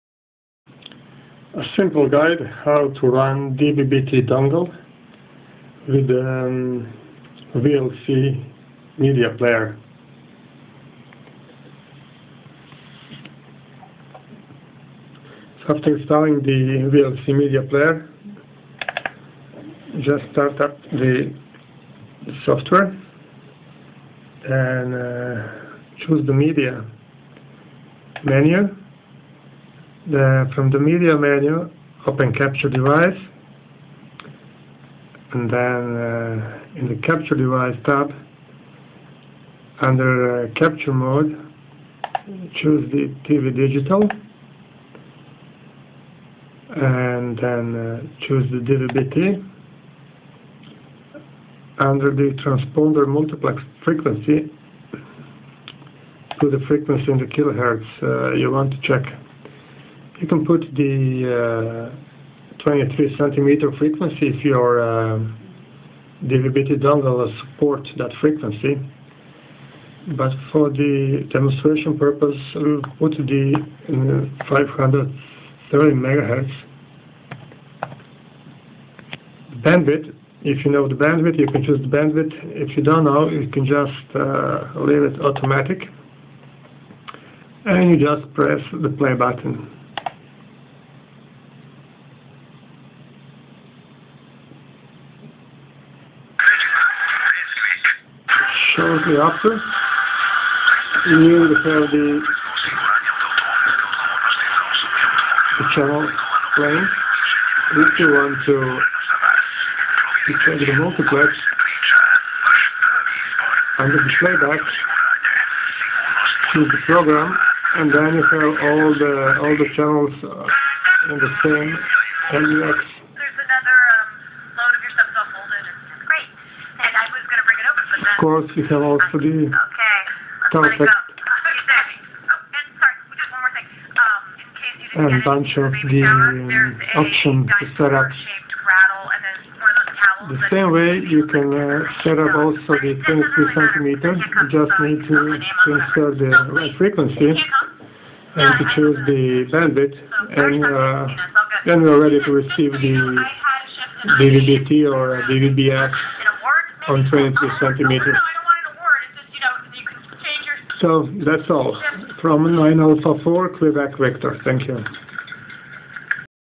VLC playing RTL2832 dongle on DVB-T signal
Watching DVB-T using the VLC player is a simple task.
You can also watch the 23cm DVB-T transmission if your hardware (DVB-T dongle) can tune the desired frequency.
All done using the orriginal dongle driver. No modification required.